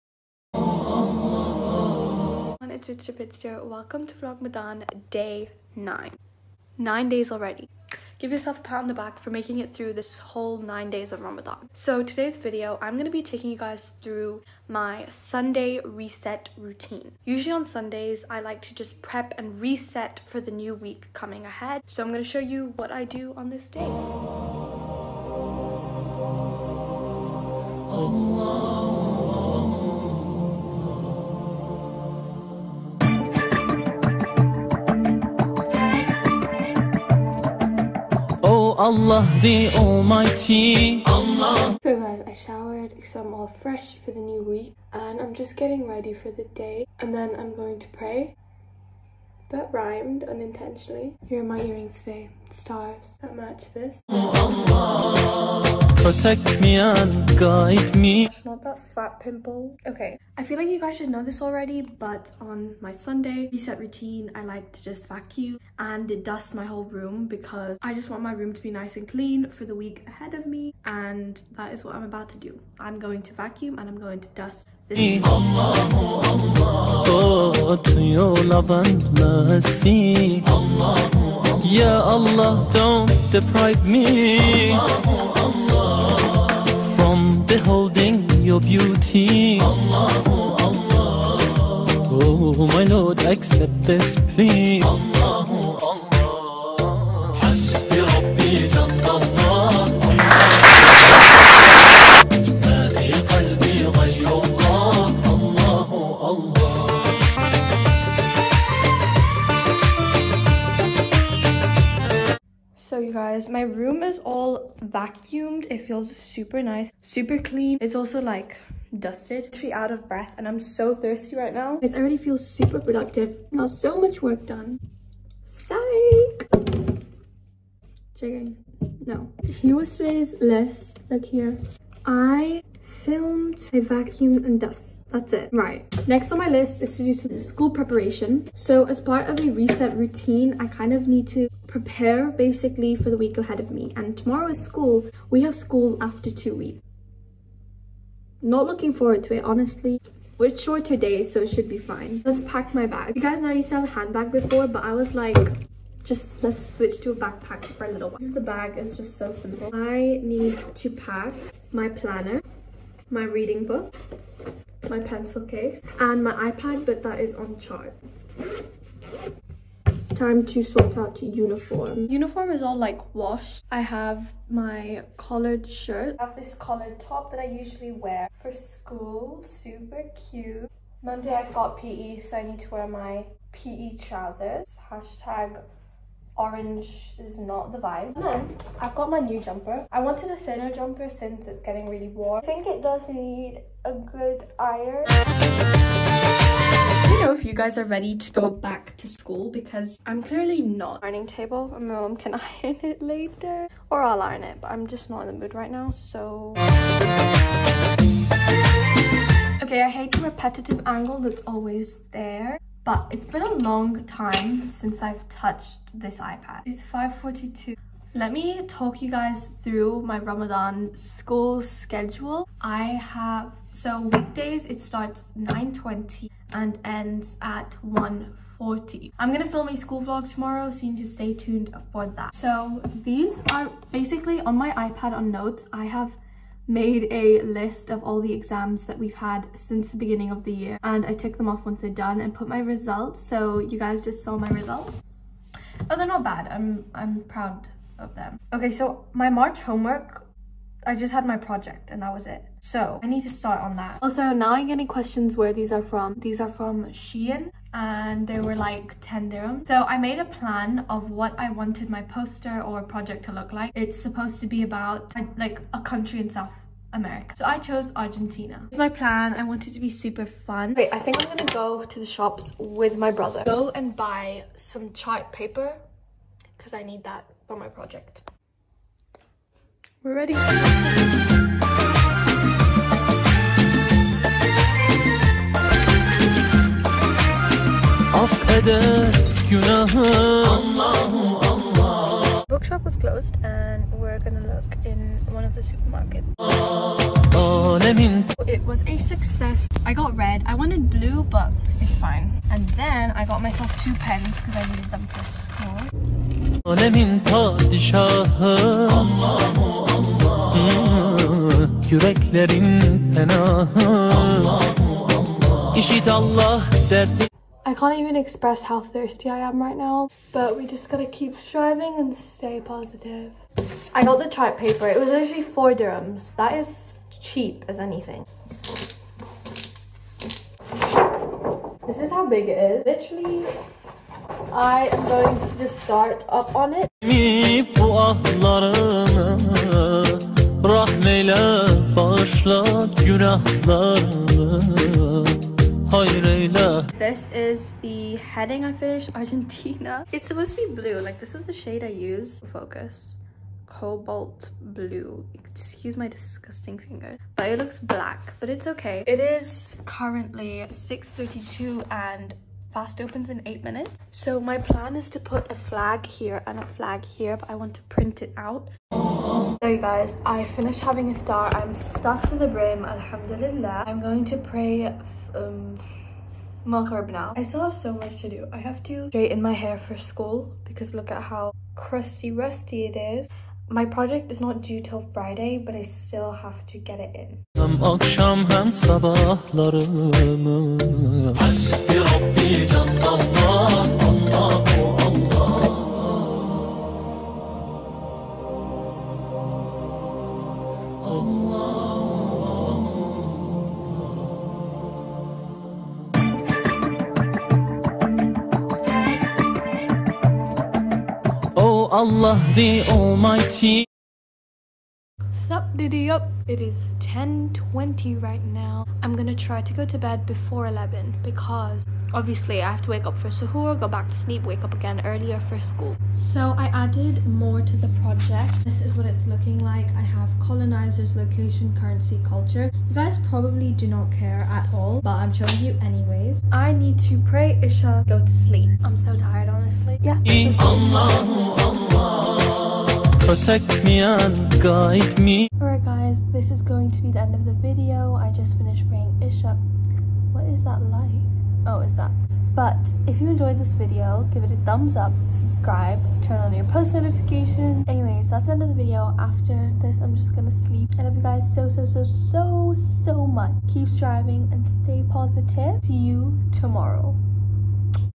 SUNDAY RESET ROUTINE| VLOGMADAN DAY 9| RIDSTER PIDSTER
Hey everyone🥰🫀

Today, I filmed a little Sunday reset routine video. I took you through all the preparations I like to do for the week ahead of me.

Ilygssssssssssssssssssssm keep striving and stay positive🫀❤️🥰💕😏😘😉